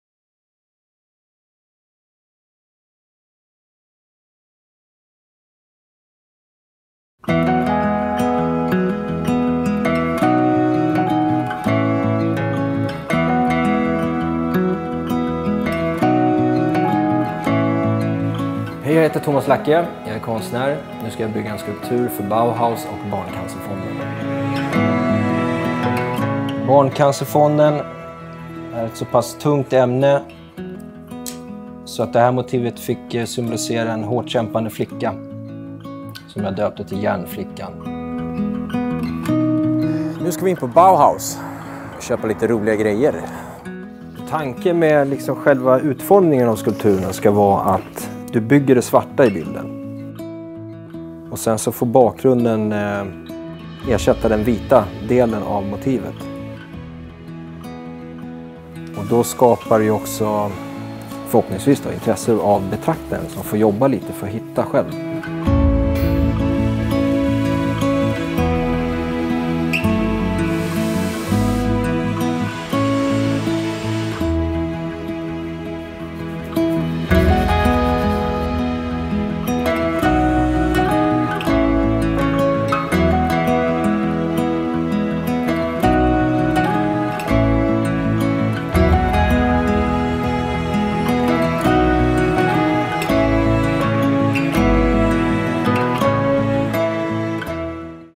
Bauhaus bygger idrott och hälsa - Barncancerfonden
Konstnären Tomas Lacke bygger en skulptur som symboliserar Bauhaus samarbete med Barncancerfonden.
Titel: Järnflickan
Konstnär: Tomas Lacke